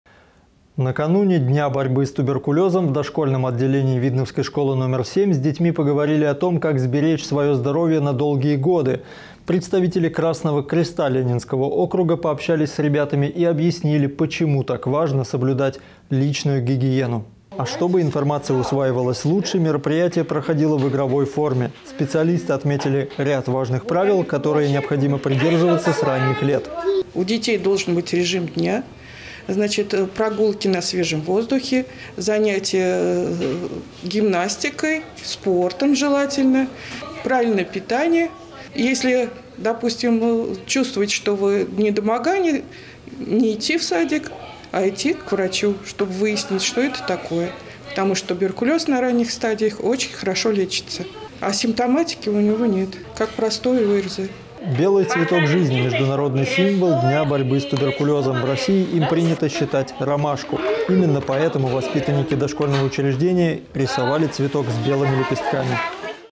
БЕЛЫЙ ЦВЕТОК ЖИЗНИ
Накануне Дня борьбы с туберкулёзом в дошкольном отделении видновской школы № 7 с детьми поговорили о том, как сберечь свое здоровье на долгие годы. Представители Красного креста Ленинского округа пообщались с ребятами и объяснили, почему так важно соблюдать личную гигиену.